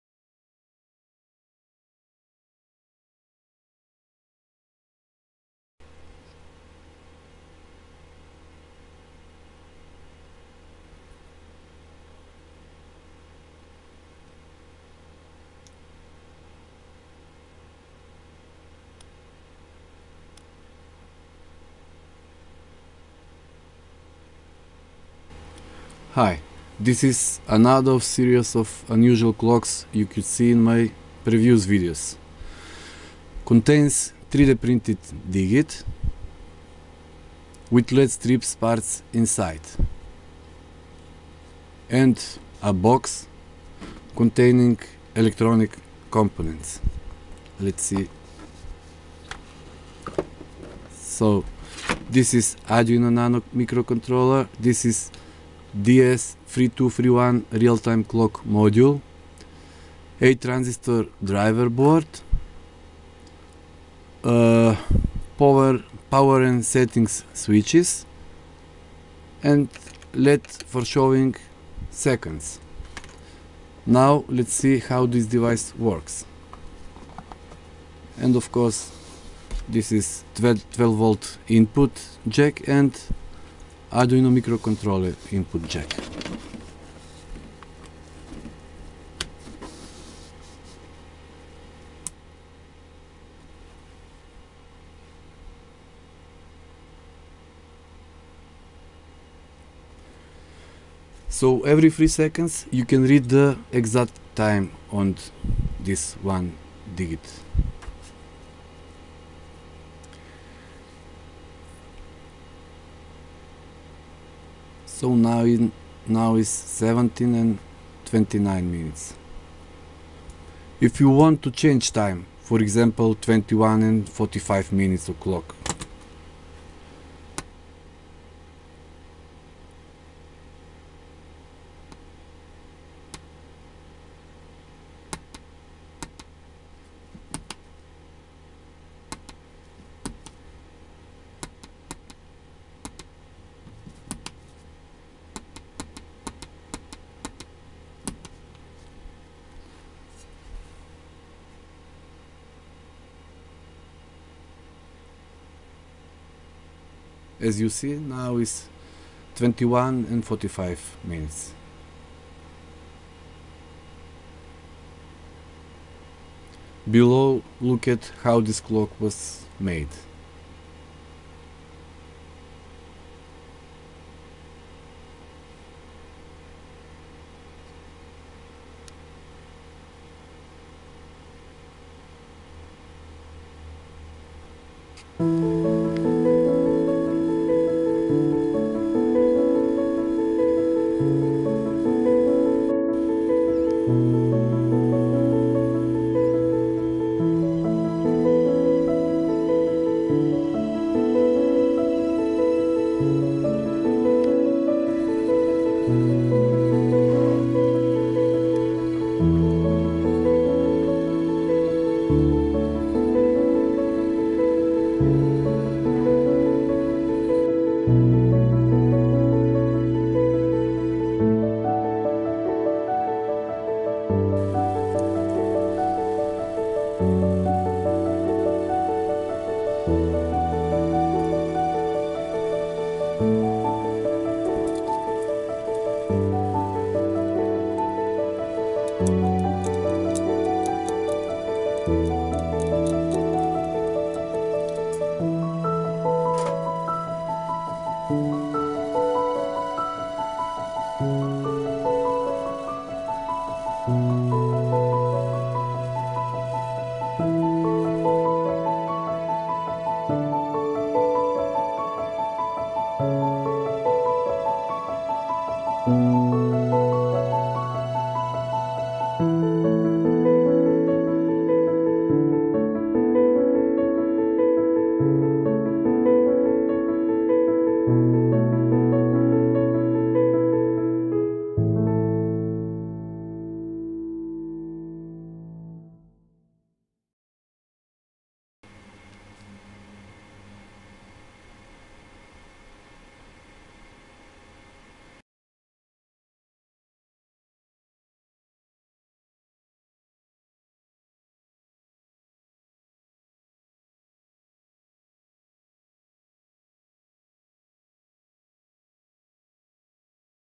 DIY 3D Printed Single Digit Arduino clock
One Big digit , fully functional Arduino Nano clock

   Another of a series of unusual clocks this time made with the help of a 3D printer.
   To build this project we need the following components:
    - Arduino Nano microcontroller board
    - DS3231 Realtime clock module
    - SMD5050 Led strip
    - 8x 2N2222 or similar Transistors
    - 8x 560 Ohm Resistors
    - 2X buttons
    - Led diode and 220 Ohm Resistor
   The code is modified for DS3231 realtime clock which is also inexpensive but much more accurate thanDS1307. The LED strip can be cut on every third  diode in series. In this case we should cut it on every second diode. For this purpose was made a small modification  that you can see in the video. Each segment of the strip is driven by a 2N2222 or similar low power transistor.
  For settings time we use two buttons.  They are connected to digital pins eight and nine (with 10k pull-down resistors). The LED display segments a~g are connected to Arduino digital pins 0~6 respectively. The decimal point is connected to the pulse output pin of the DS3231 – which will be set to a 1Hz output to have a nice constant blinking LED to show the clock is alive and well.
  Arduino and other electronics are housed in a convenient box with a 7 segment display above. Below is a link where you can download the code and .stl files for 3D Print.